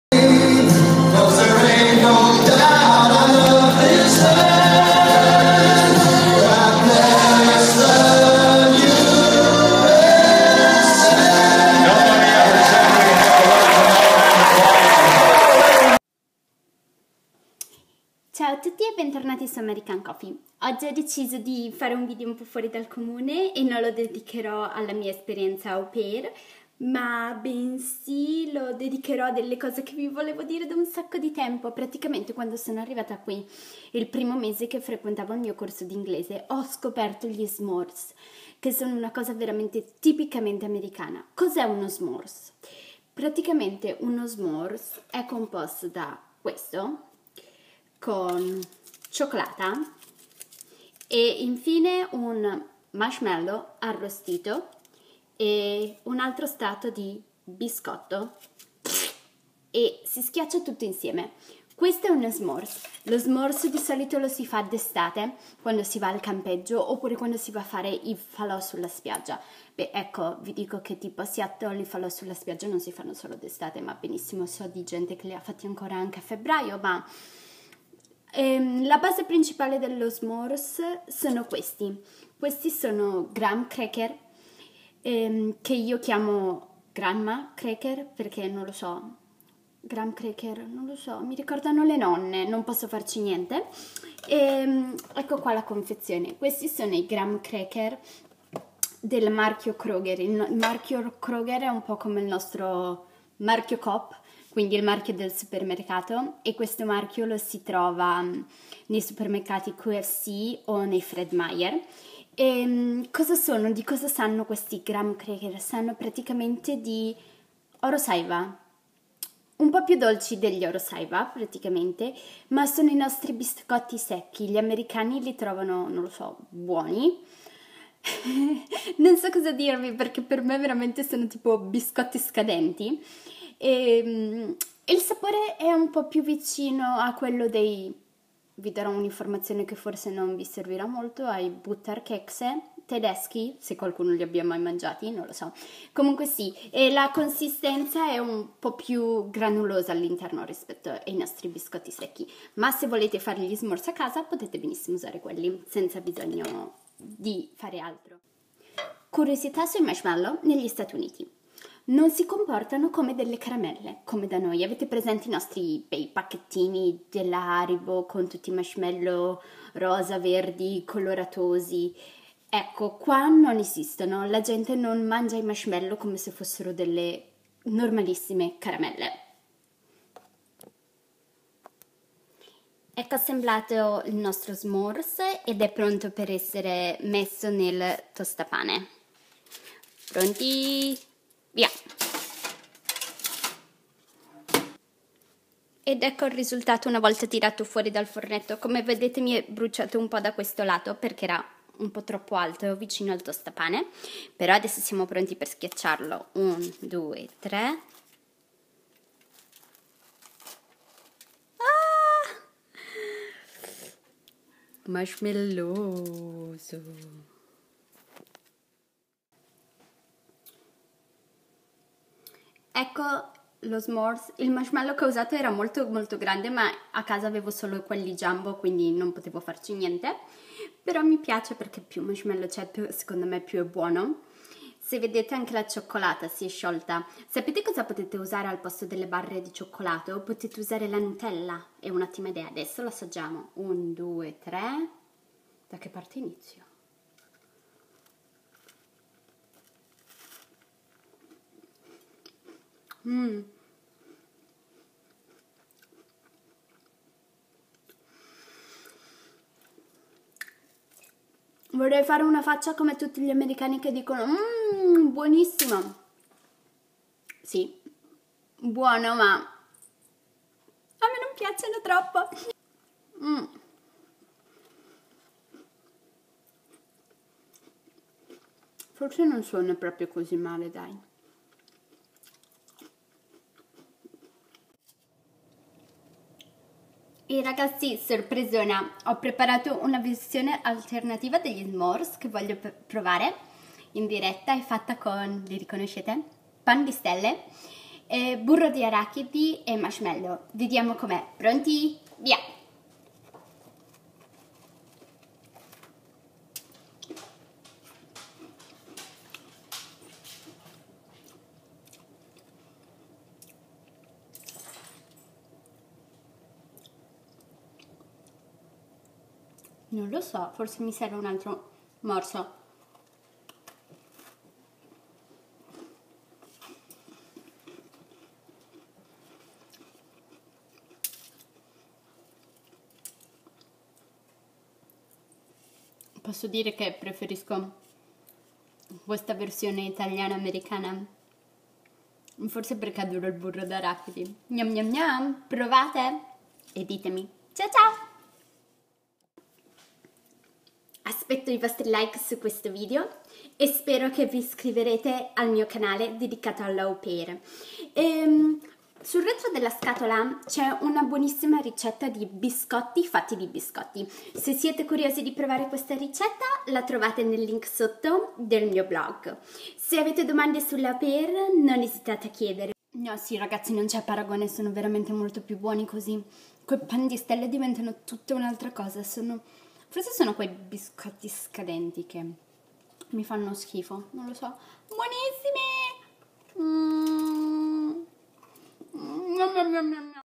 # 17AC au pair in america - S'mores
Parliamo di cibo oggi ed in particolare facciamo gli S'mores !
Ingredienti principali biscotti, cioccolato e marshmallow, super calorici e golosi. gli S'mores si fanno sopratutto in campeggio e durante i falò sulla spiaggia, ma se vie viene voglia anche in un altro momento basta metterli nel fornetto arrostire e poi schiacchiare ! Provate entrambe le versioni che vi propongo e ditemi la vostra preferita nei commenti :) Ecco a voi la ricetta si biscotti fatti di biscotti